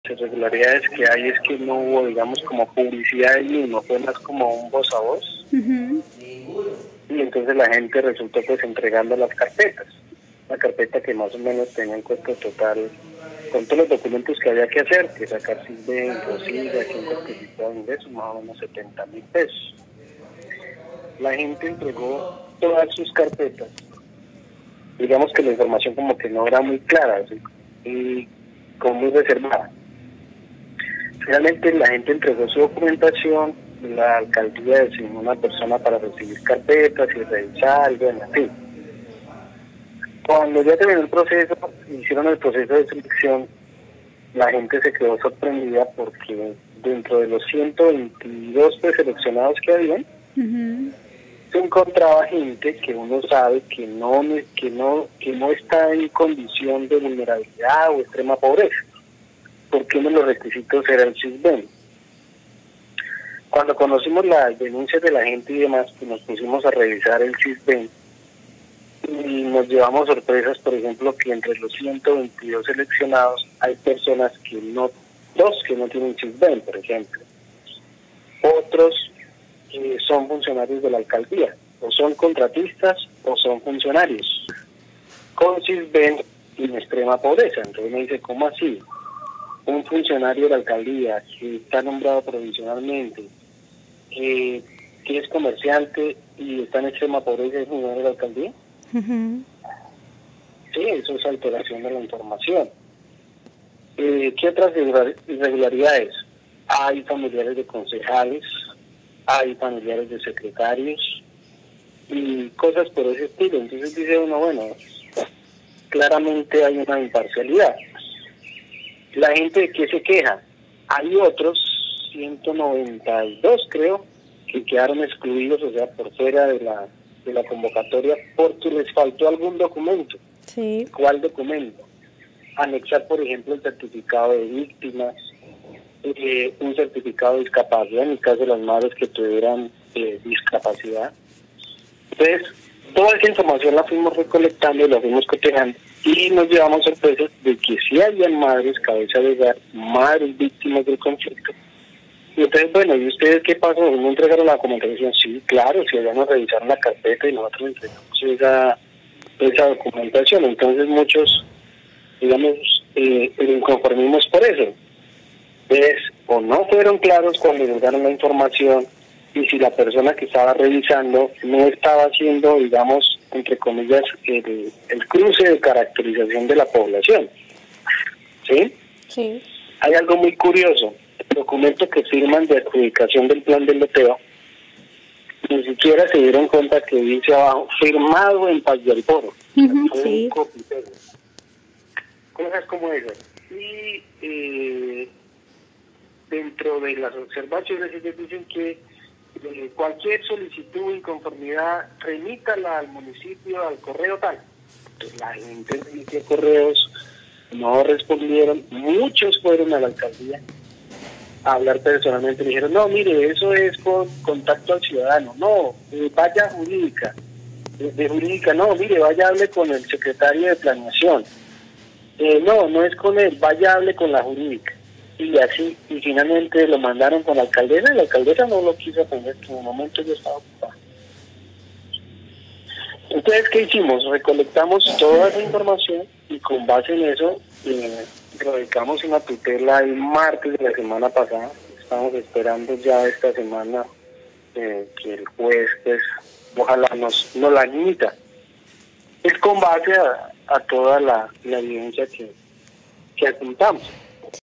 TUTELA CONTRA LA ALCALDÍA DE PORE POR POLÉMICA ASIGNACIÓN DE VIVIENDA
Centenares de personas dicen que fueron excluidos para acoger a candidatos que no cumplen requisitos